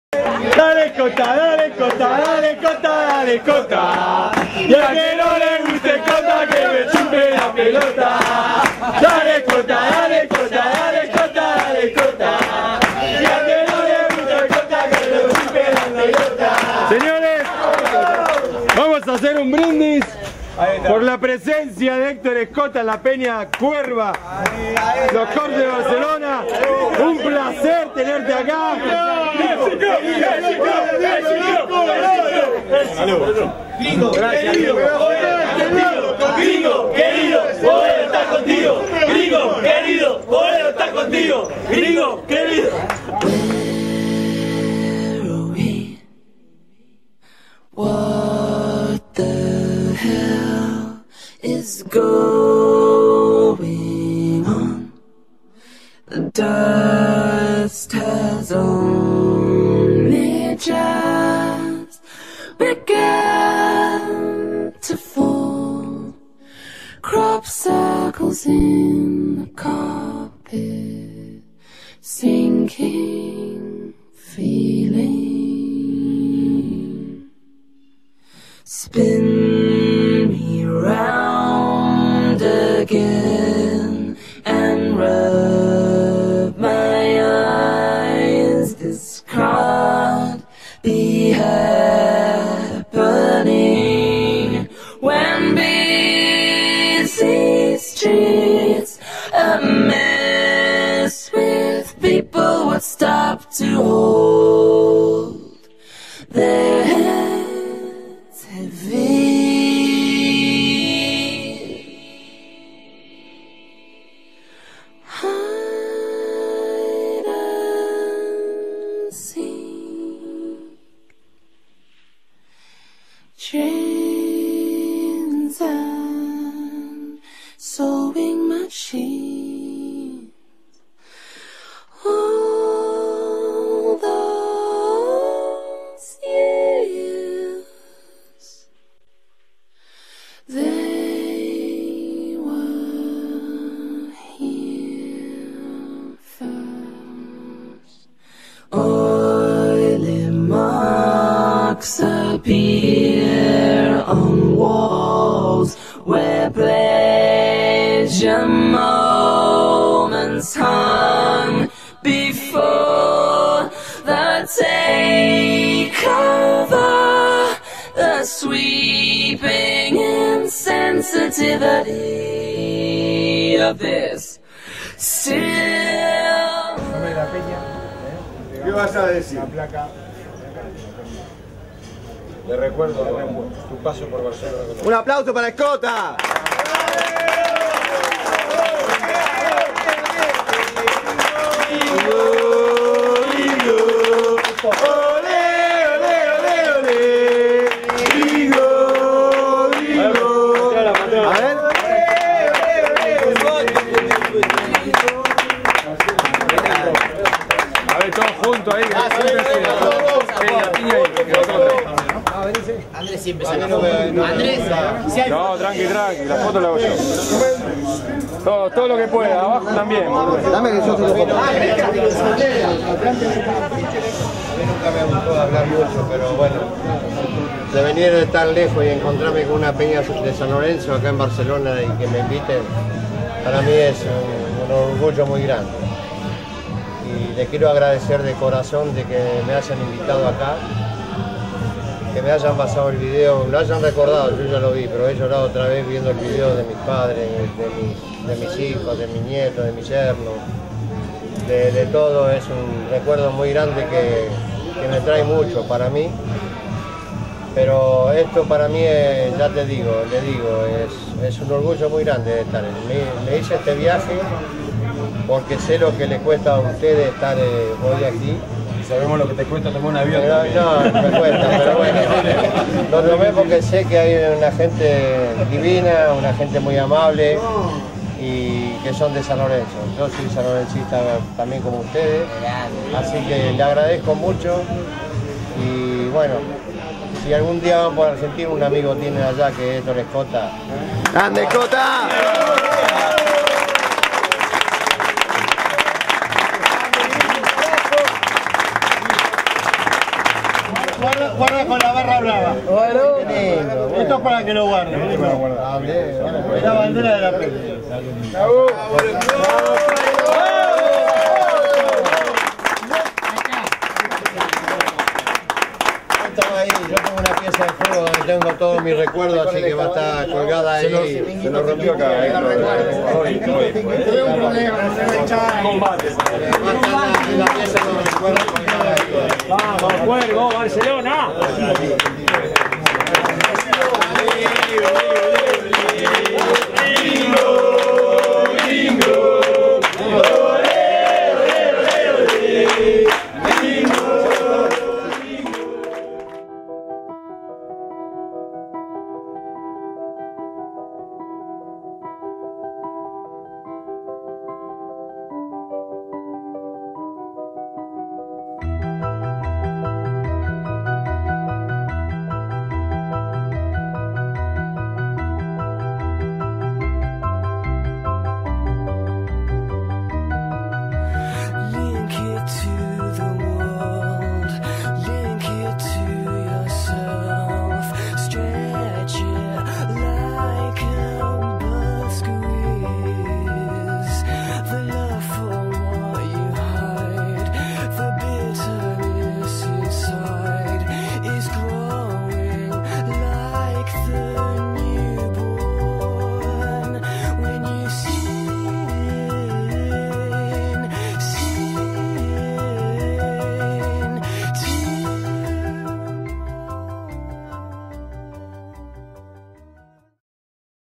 Homenaje al GRINGO SCOTTA (Parte 2) - Los CORBS de Barcelona -
Segunda parte de un día de gloria para la Peña Corbs, de Barcelona. Noche histórica para la historia de San Lorenzo de Almagro.